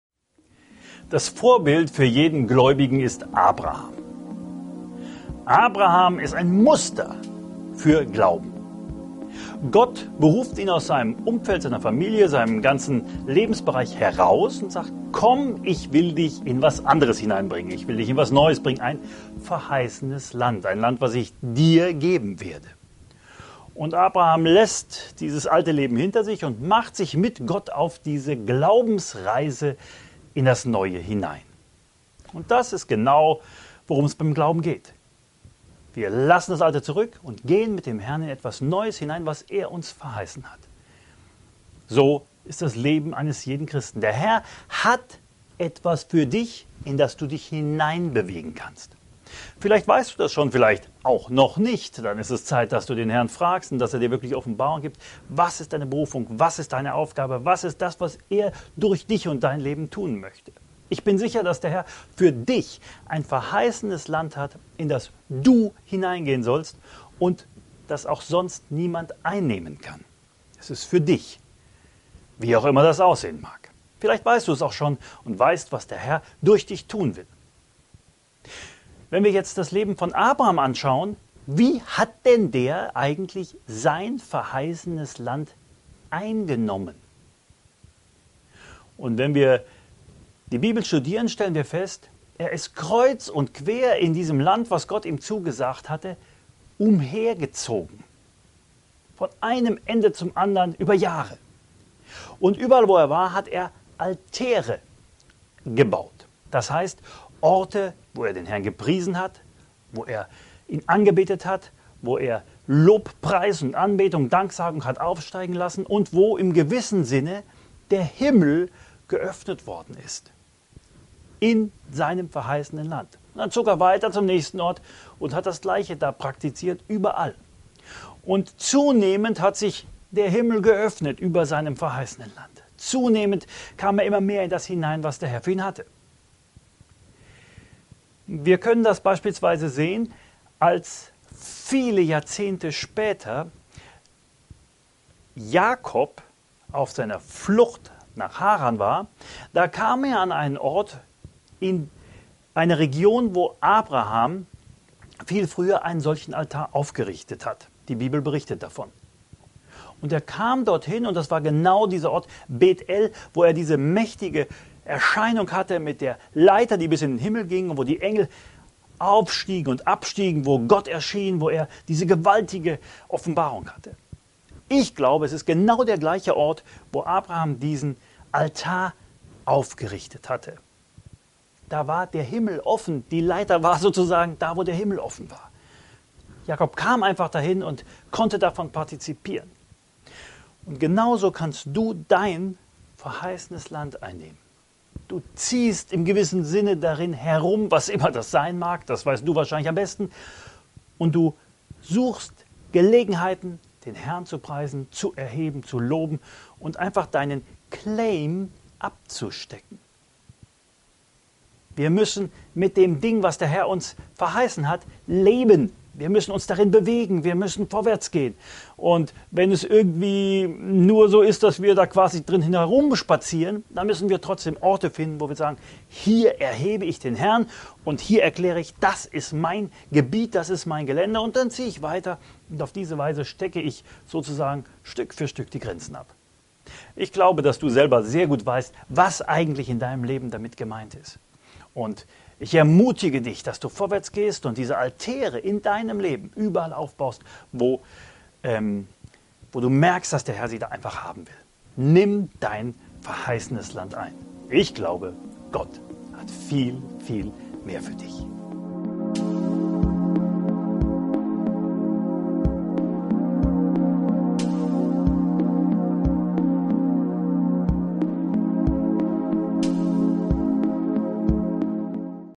Wie du dein verheißenes Land einnimmst
Das Vorbild für jeden Gläubigen ist Abraham. Er ließ sein altes Leben hinter sich und machte sich mit Gott auf eine Glaubensreise in das Neue hinein auf, das der Herr ihm verheißen hatte. So ist das Leben jedes gläubigen Christen. Der Herr hat ein verheißeneres Land für dich, zu dem hin du unterwegs bist. Doch was tat Abraham eigentlich, um sein Land einzunehmen?

Abraham zog in seinem verheißenen Land umher und steckte damit gewissermaßen seinen „Claim" ab. Er durchzog es kreuz und quer. Jahrelang. Und er errichte Altäre als Orte der Danksagung, des Lobpreises und der Anbetung für Gott. An diesen Orten öffnete er dadurch in einem gewissen Sinne den Himmel -- und kam zunehmend mehr er in das hinein, was der Herr ihm verheißen hatte.

Einige Bibelstellen zum Thema Altäre aufrichten:
1.Mose 12,7-8; 13,4; 13,18; 26,25; 28,11-19; 33,20; 35,7

Und der HERR sprach zu Abram: Geh aus deinem Land und aus deiner Verwandtschaft und aus dem Haus deines Vaters in das Land, das ich dir zeigen werde! Und ich will dich zu einer großen Nation machen, und ich will dich segnen, und ich will deinen Namen groß machen, und du sollst ein Segen sein! Und ich will segnen, die dich segnen, und wer dir flucht, den werde ich verfluchen; und in dir sollen gesegnet werden alle Geschlechter der Erde!
1.Mose 12,1-3

Und der HERR erschien dem Abram und sprach: Deinen Nachkommen will ich dieses Land geben. Und er baute dort dem HERRN, der ihm erschienen war, einen Altar. Und er brach von dort auf zu dem Gebirge östlich von Bethel und schlug sein Zelt auf, Bethel im Westen und Ai im Osten; und er baute dort dem HERRN einen Altar und rief den Namen des HERRN an.
1.Mose 12,7-8

Und er fürchtete sich und sagte: Wie furchtbar ist diese Stätte! Dies ist nichts anderes als das Haus Gottes, und dies die Pforte des Himmels. ... Und er gab dieser Stätte den Namen Bethel.
1.Mose 28,11-19

Und die Schrift wurde erfüllt, welche sagt: Abraham aber glaubte Gott, und es wurde ihm zur Gerechtigkeit gerechnet, und er wurde Freund Gottes genannt.
Jakobus 2,23